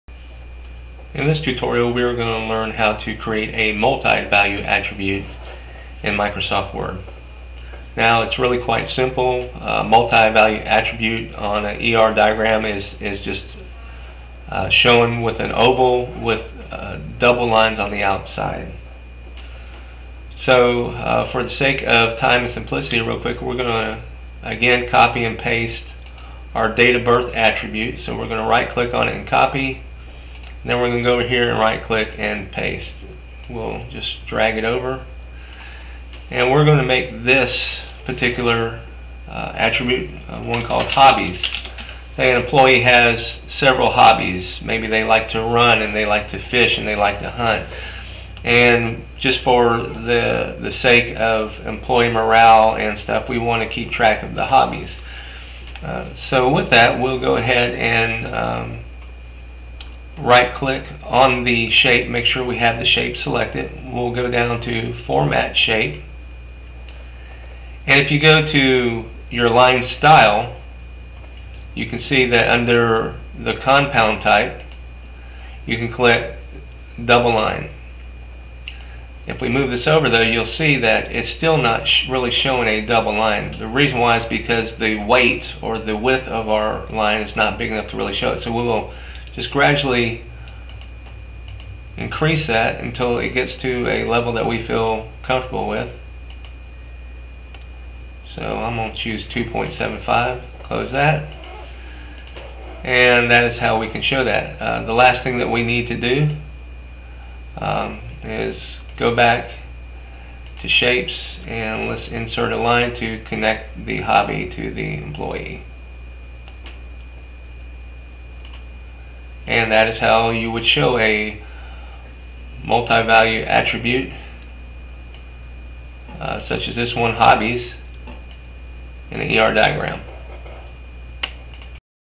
ER Diagram in MS Word Part 4- Creating A Multivalued Attribute
This tutorial explains how to create and illustrate a multivalued attribute for an ER diagram using Microsoft Word.